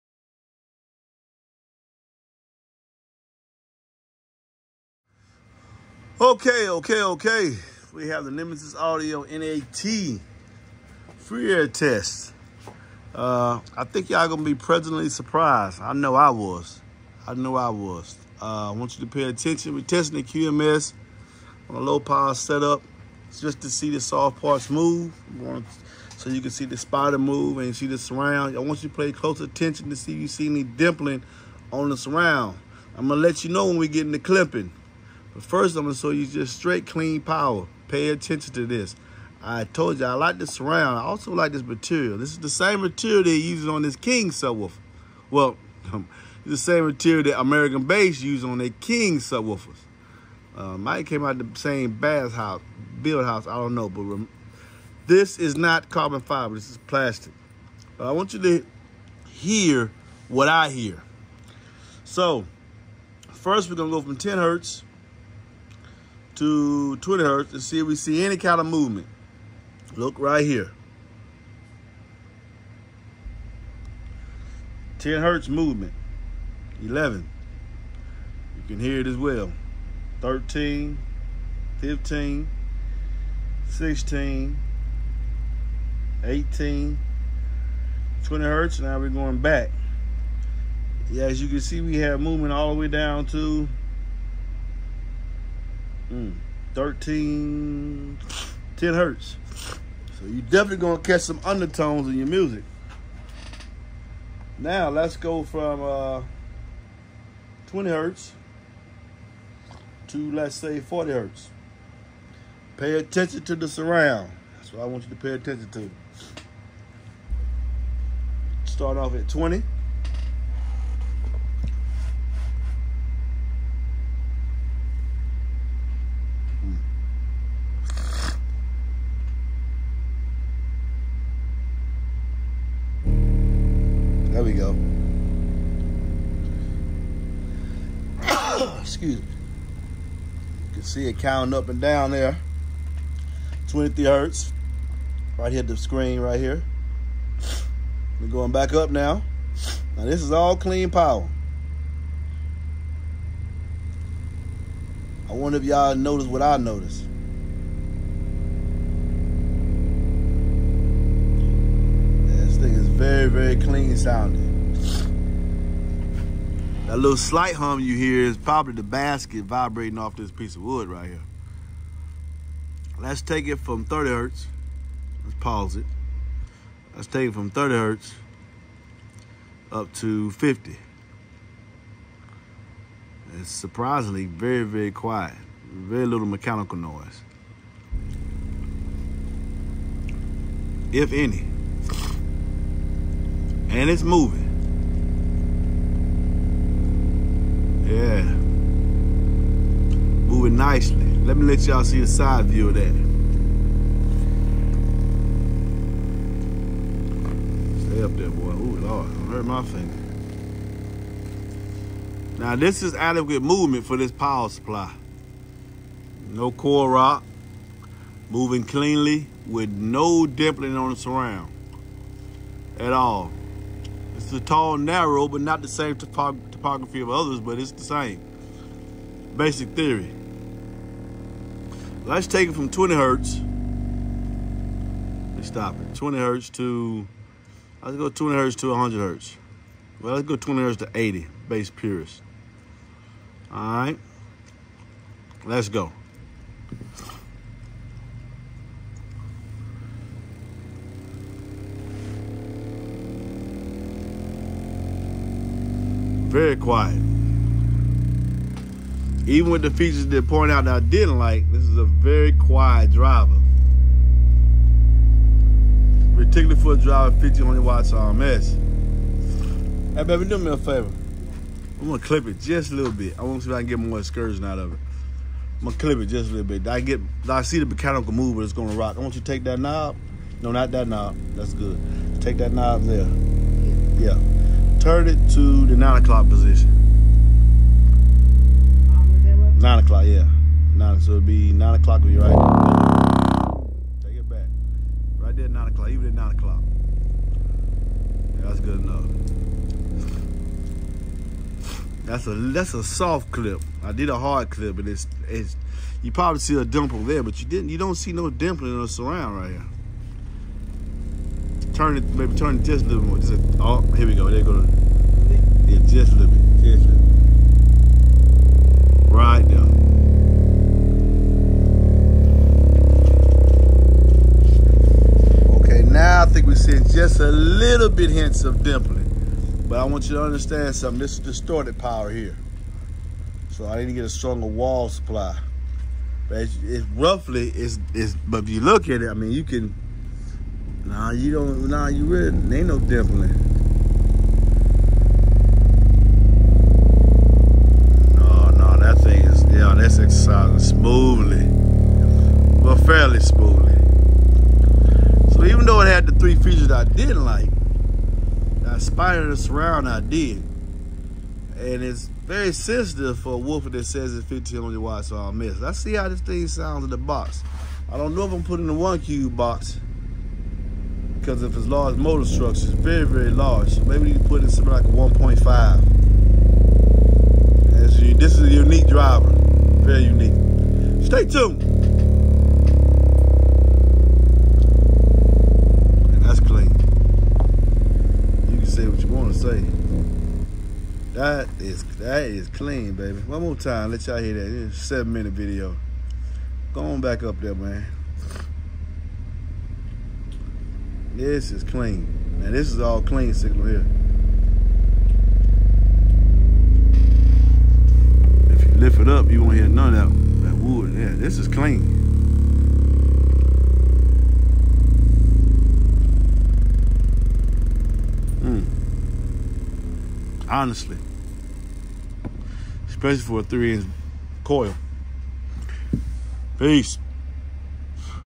Nemesis Audio NA-8T Free Air Frequency Sweep!! No Mechanical Noise and Serious Excursion!!
To order and enclosure for this sub please call or text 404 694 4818